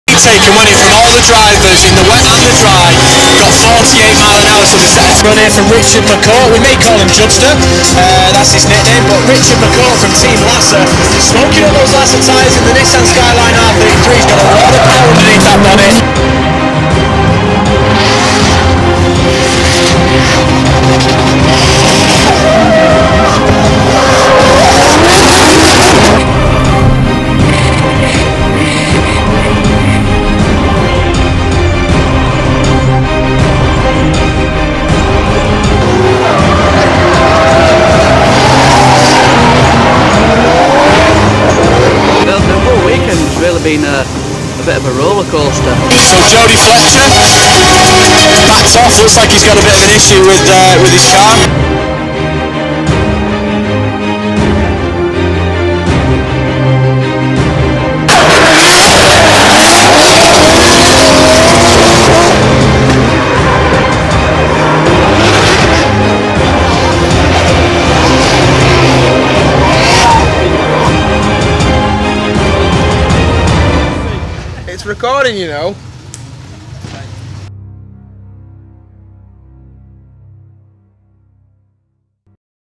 Lassa Tyres Drift Team BDC Round 3 Teaser
Working on the high and lows of the weekend for Lassa Tyres Drift Team on the track at Teeside British Drift Championships Round 3, Here is what's to come :)